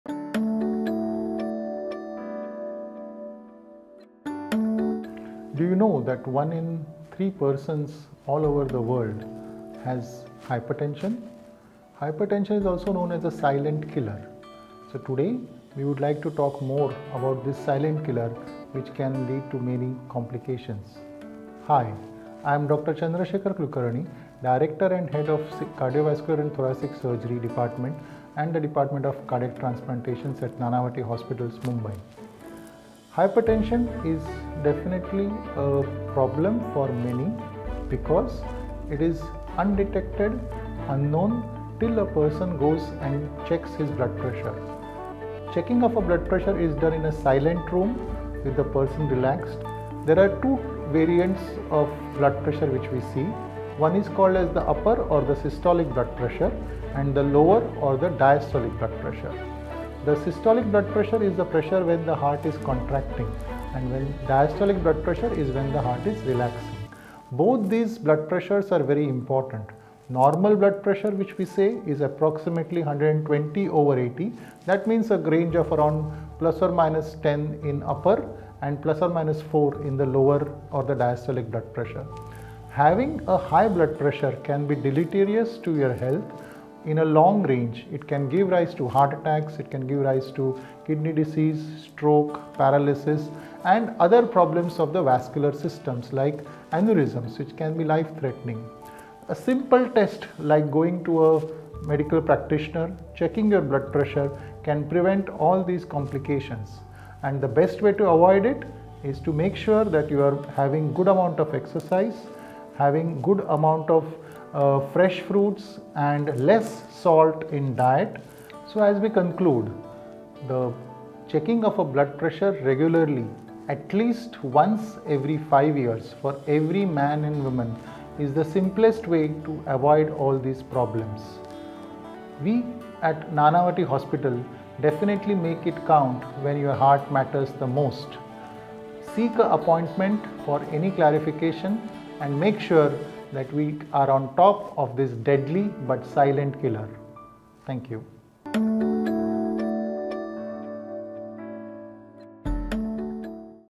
Understanding Hypertension │ Dr. Chandrashekhar Kulkarni│ Nanavati Max Hospital, Mumbai
This WorldHypertensionDay, let’s understand how high blood pressure impacts your heart and overall health. Join Dr. Chandrashekhar Kulkarni, Director & Head, CTVS and Heart & Lung Transplant, Nanavati Max Hospital, Mumbai, as he explains clinical terms like 'systolic' and 'diastolic' blood pressure and offers valuable insights on managing hypertension. Don't miss this opportunity to enhance your knowledge and take proactive steps towards better health.